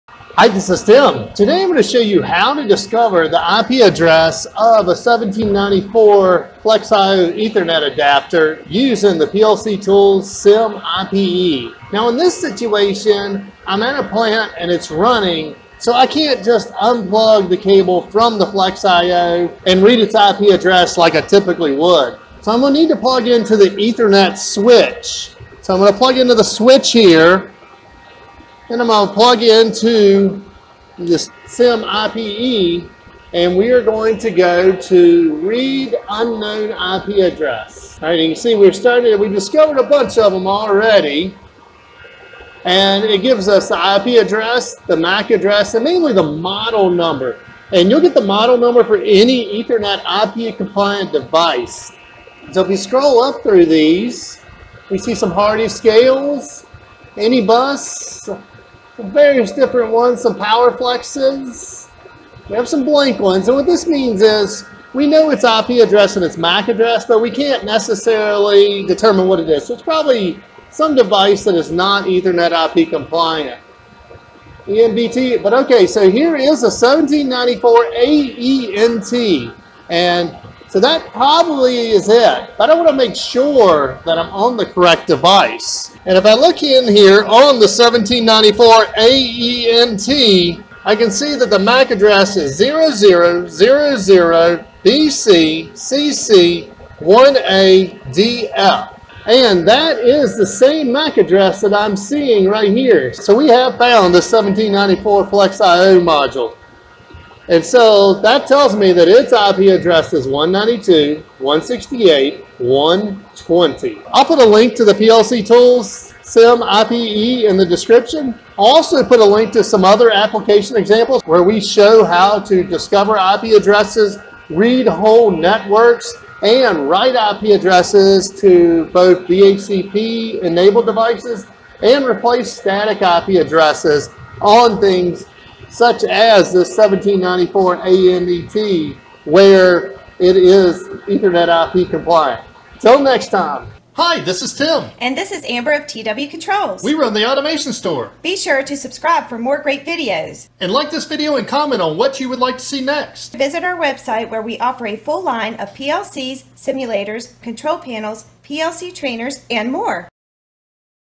Discovering the IP Address of an Allen Bradley 1794-AENT Flex I/O Module
You can discover the IP address of an Allen Bradley 1794-AENT Flex I/O module using the PLC Tools SIM-IPE by either plugging directly into the modules Ethernet port or plugging into a spare port on the Ethernet switch that the 1794-AENT is connected to and verifying you have the correct device by checking it's MAC address.